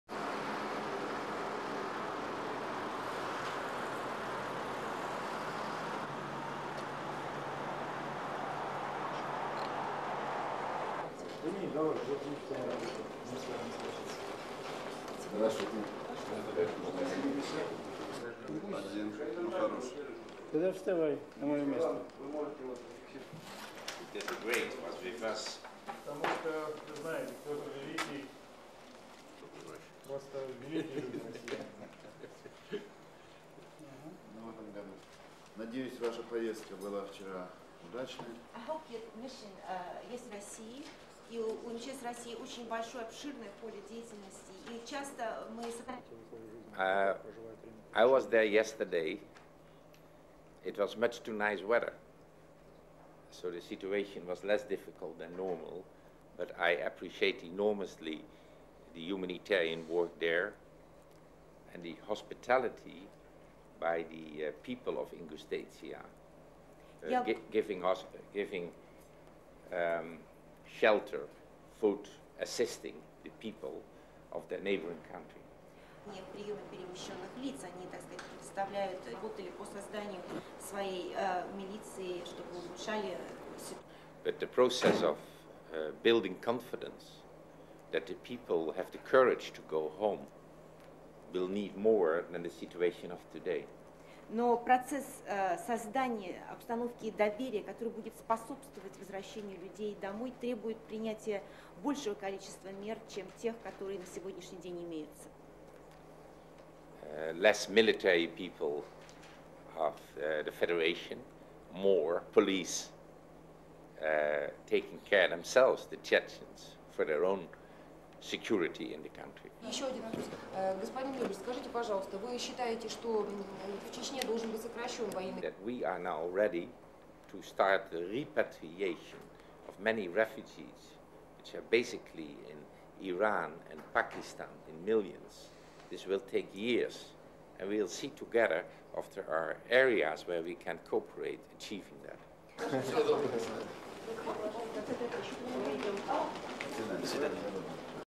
UNHCR chief in talks on Chechnya refugees
1. Exterior Ministry for Emergency Situations (MES) of Russia
2. Sign "Ministry for Emergency Situations"
3. UN High Commissioner for Refugees (UNHCR) Ruud Lubbers walks in, greets MES officials
4. Camera
5. Round-table shot of Lubbers and MES officials
6. Lubbers
7. Round-table
8. Lubbers at front of press briefing
9. SOUNDBITE: (English) Ruud Lubbers, UNHCR
"I was there (Ingushetia) yesterday. It was nice weather, so the situation was less difficult than normal. But, I enormously appreciate the humanitarian work there and the hospitality by the people of Ingushetia giving shelter, food and assisting the people of their neighbouring country."
10. Cut away Journalist
11. Lubbers at front of press briefing
12. SOUNDBITE: (English) Ruud Lubbers, UNHCR
"The process of building confidence so that the people have the courage to go home will need more than the situation of today. (We need) Less military people of the Federation, more Chechen police taking care themselves for their own security in the country." (pauses while translator speaks)
13. SOUNDBITE: (English) Ruud Lubbers, UNHCR
"We are now ready to start the repatriation of many refugees who are now in Iran and Pakistan in millions. This will take years. And, we will see together what are the areas where we can cooperate achieving that."
14. Officials leave press briefing

STORYLINE:

The UN High Commissioner for Refugees Ruud Lubbers on Thursday started a round of talks with Russian officials in Moscow aimed at resolving the plight of Chechen refugees languishing in camps in the republic of Ingushetia.

The talks came one day after Lubbers toured Ingushetia refugee camps, where displaced people from Chechnya are spending their third winter.

On Thursday morning, Lubbers met with officials from the Emergency Situations Ministry and with Interior Minister Boris Gryzlov, who is in charge of Russia's police and several troop units in Chechnya.

Talking to journalists after the meeting, Lubbers acknowledged the dire conditions in the camps and thanked Russian humanitarian workers for providing vital assistance to the refugees.

He added that the crucial talks are aimed at building up the confidence of the refugees so they will deem it safe to return to their homes in Chechnya.

Lubbers insisted that ethnic Chechen -- not Russian -- police should carry the responsibility of enforcing order in the republic.

Touching on the subject of Afghan refugees living in Iran and Pakistan, Lubbers said that the UNHCR and the Russian Ministry for Emergency Situations will cooperate in helping refugees to go back home to Afghanistan.